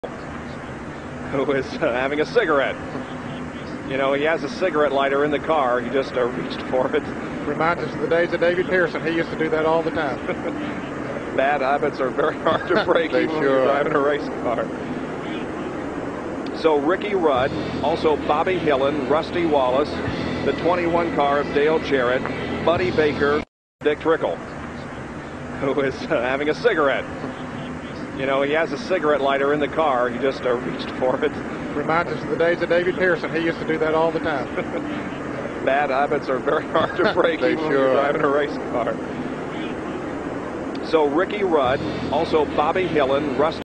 NASCAR WHAT? Dick Trickle smoking cigarettes in the middle of A Race
Dick Trickle actively smoking cigarettes in the middle of Nascar Race 🥂